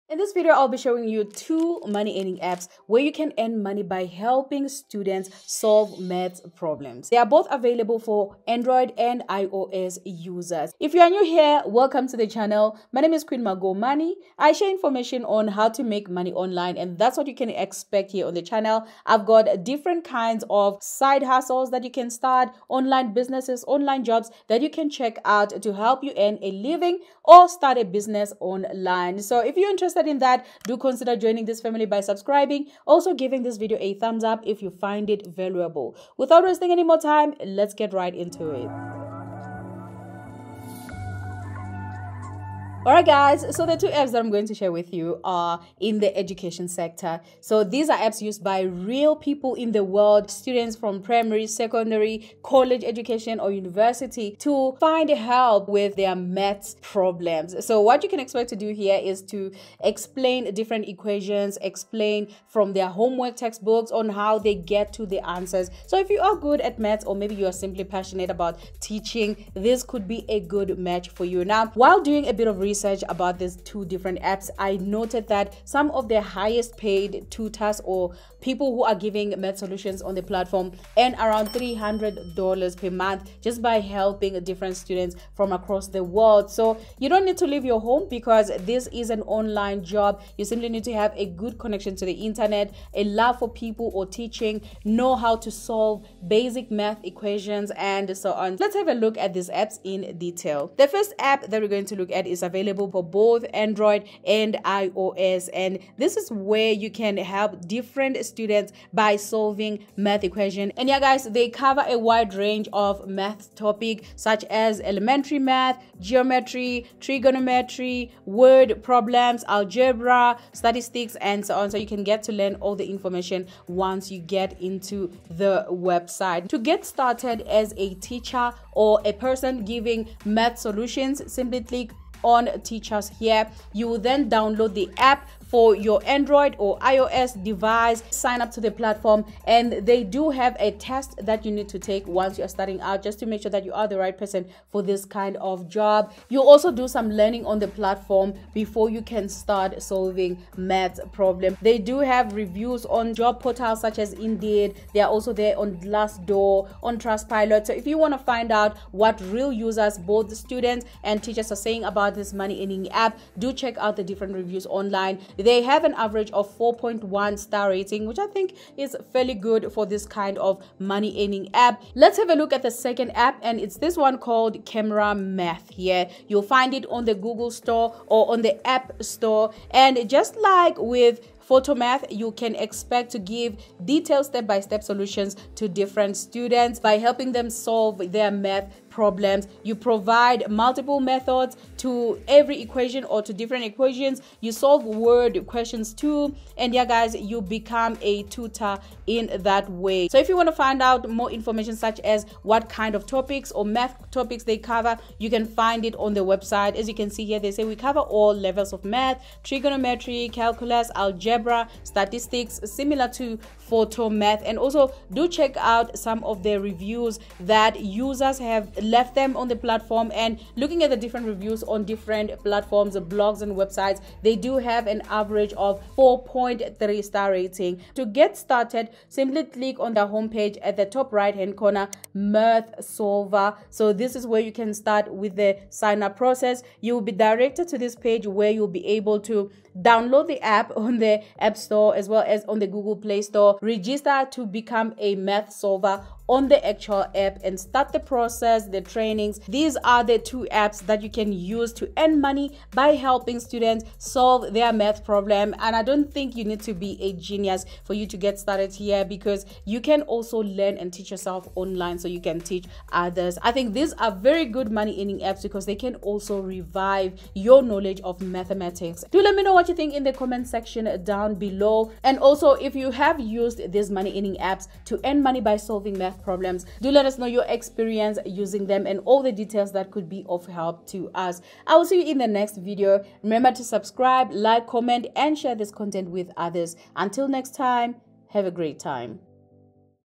2 New Earning Apps to Make Money Today by Solving Math Problems
Did you know you can make money online by solving Math equations? In this video, I share 2 new apps where you can get paid to give math answers to students worldwide in 2022.